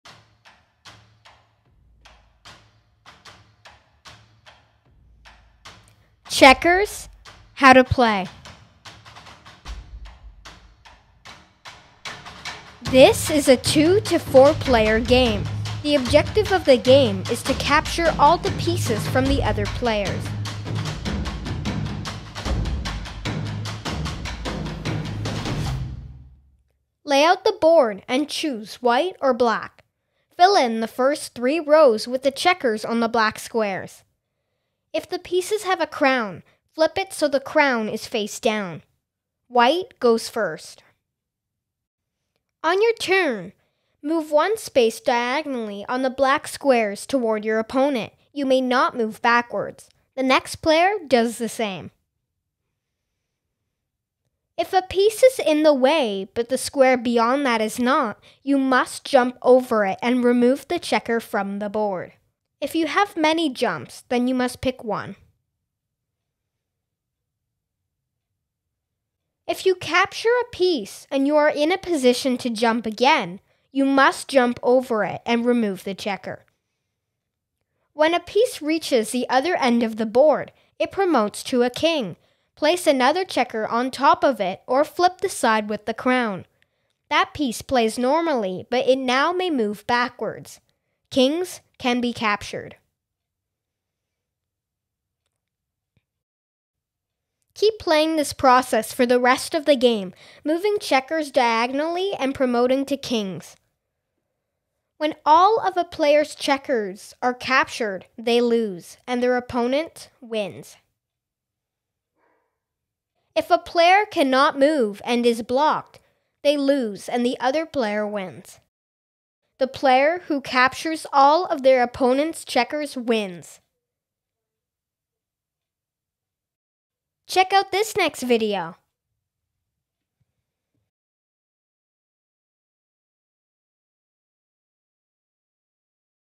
How to play Checkers
Don't have this game?
This is a fast and easy video to follow on How to play Checkers.
Instructions :
Checkers how to play.

This is a 2 player game
The objective of the game is to capture all the pieces from the other player.

Layout the board and choose white or black. Fill in the first three rows with the checkers on the black squares. If the pieces have a crown, flip it so the crown is face down. White goes first.

On your turn, move one space diagonally on the black squares toward your opponent. You may not move backwards. The next player does the same.

If a piece is in the way but the square beyond that is not, you must jump over it and remove the checker from the board.
If you have many jumps, you must pick one.

If you capture a piece and you are in a position to jump, you must jump over it again and remove the checker.

When a piece reaches the other end of the board, it promotes to king. Place another checker on top of it or flip the side with the crown. That piece plays normally but it may now move backwards. Kings can be captured.

Keep playing this process for the rest of the game moving checkers diagonally and promoting to kings.
When all of a player's checkers are captured they lose, and their opponent wins.

If a player can not move and is blocked, they lose and the other player wins.

The player who captures all of their opponent's checkers wins!